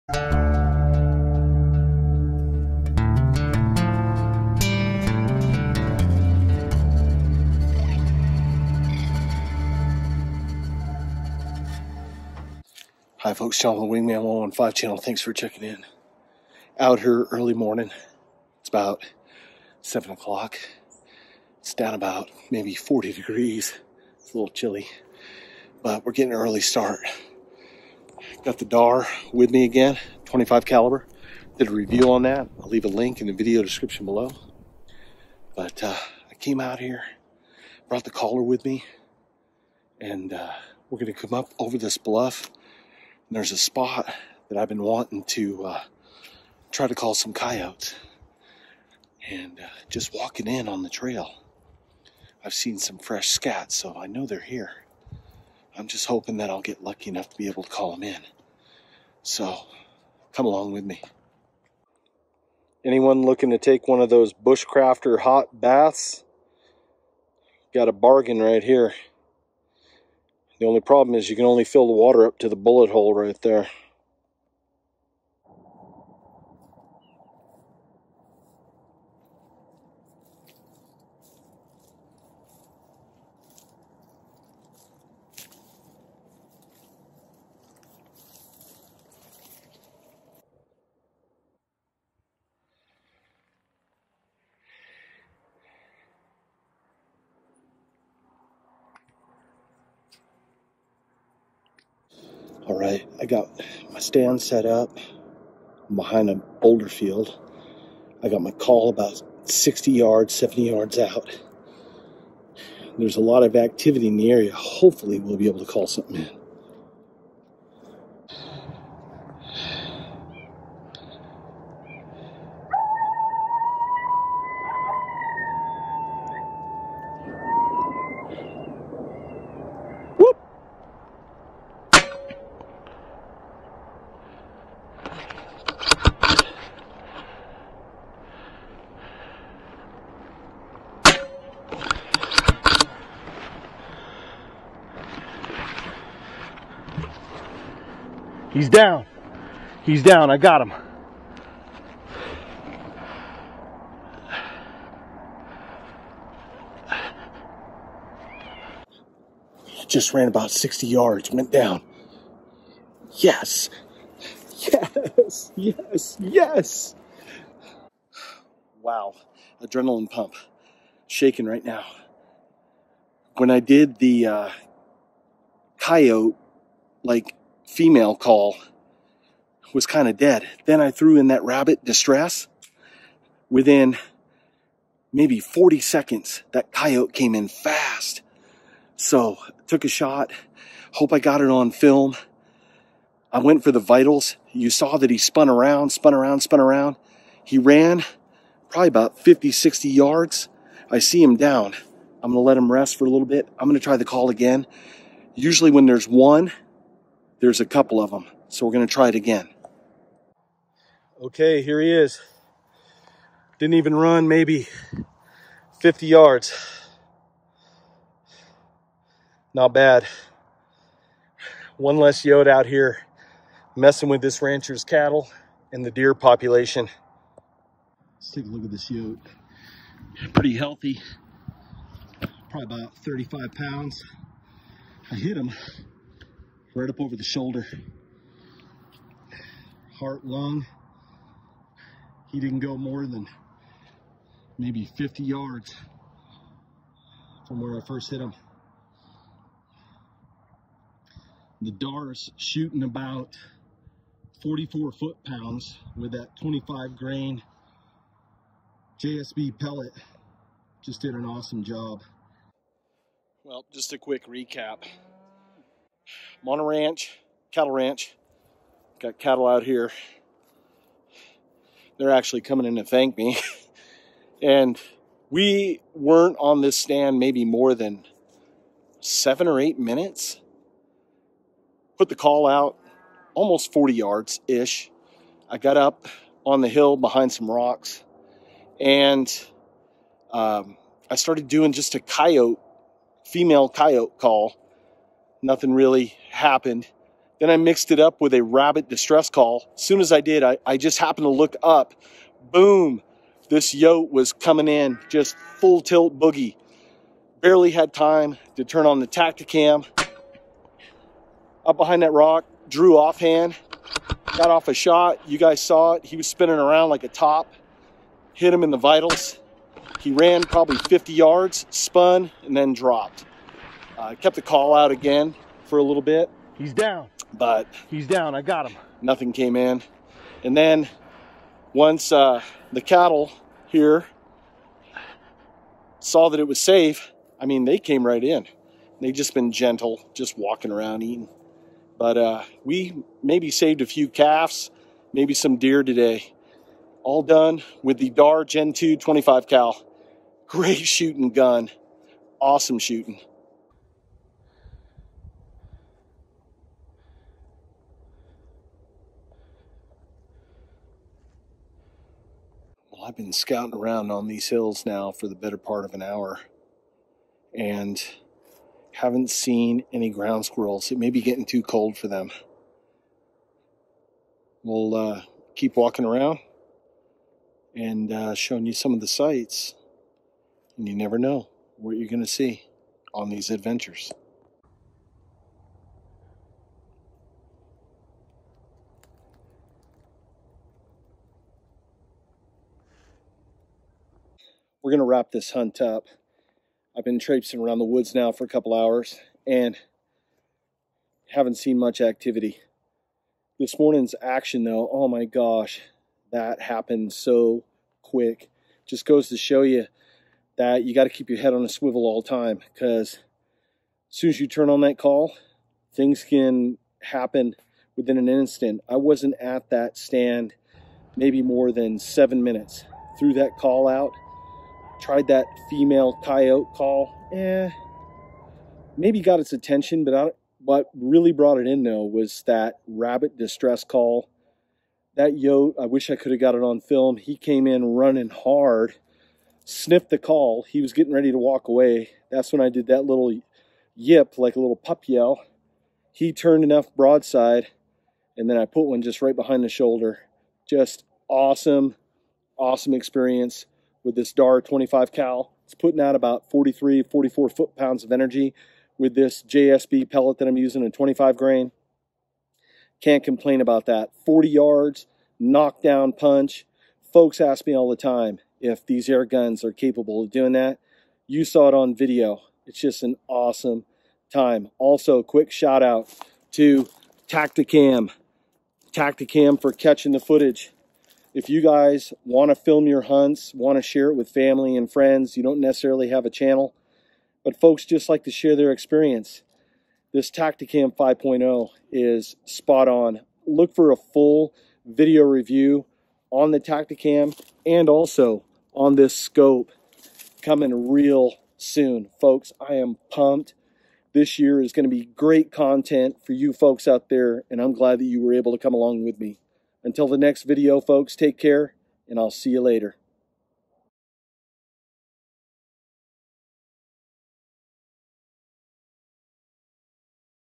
Airgun Coyote Hunt "Dog Down"
I had the chance to get out on a morning coyote hunt with my Dynamic Air Rifles DAR Gen 2 .25 Cal. I quickly get set up when the action starts. Follow along with me on this awesome airgun coyote hunt.
 The video covers the basic set up for a coyote stand and how to hunt coyotes with a airgun. First your going to need to find a prime location. Once a location is found you need to set up your gear properly to help increase the odds of calling in a coyote. In this video I used a ICOtec GC300 caller.